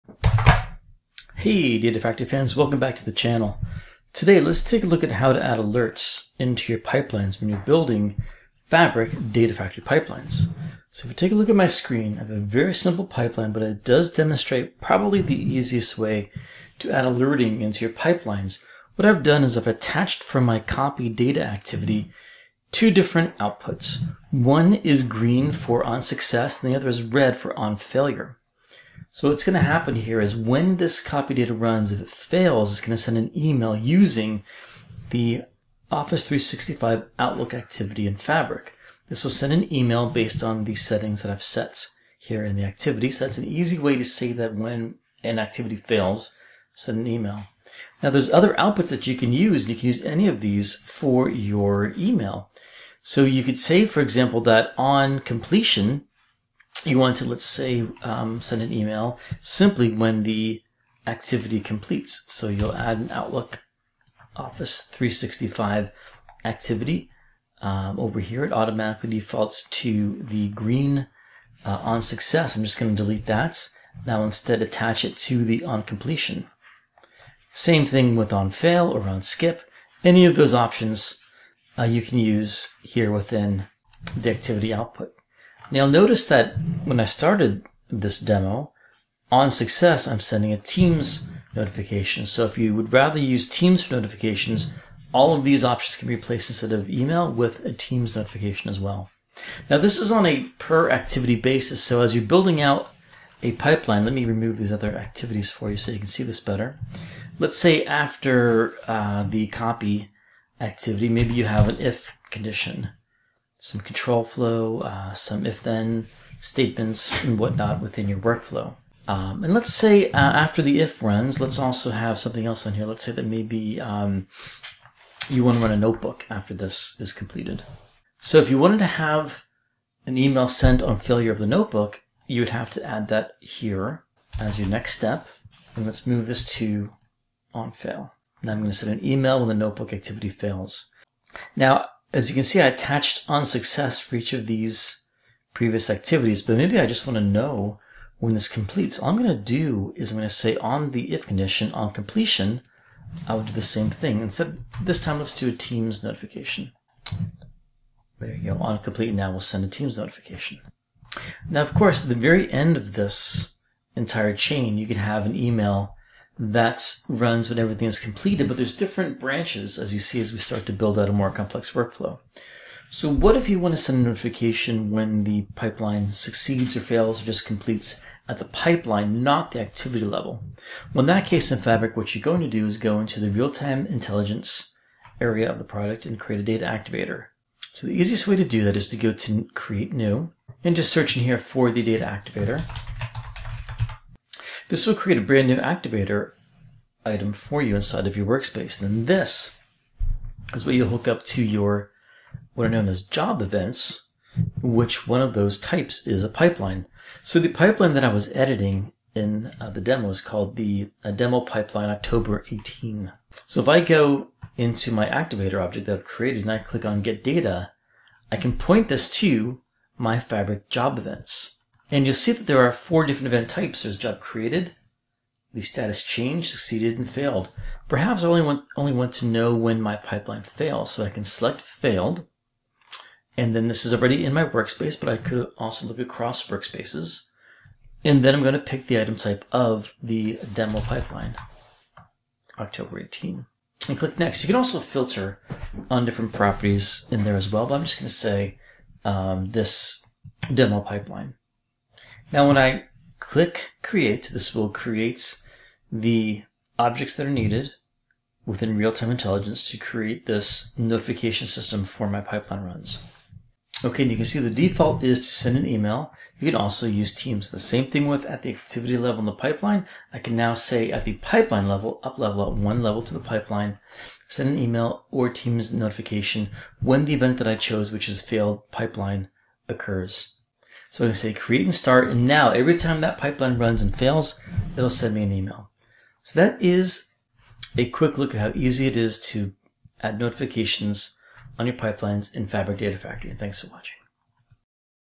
Fabric Data Factory Pipeline Notifications
In this video, Mark shows you how easy it is add notifications to your Fabric Data Factory pipelines using the Outlook and Teams activities at the activity level and using Fabric Activator at the pipeline job execution level.